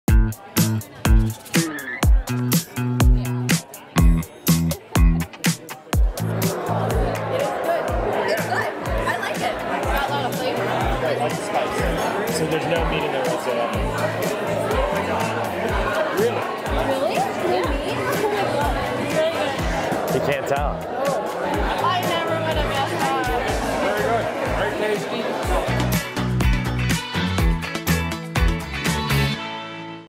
freshii - Beyond CHILii is here - 30sec
BEYOND CHILii
Perfect when you're craving something healthy, warm and hearty - 26 grams of satisfying protein in every bowl!
Vegan chili served over hot brown rice, topped with Beyond Meat crumbles, creamy Greek yogurt and green onion, served with tortilla chips for extra crunch.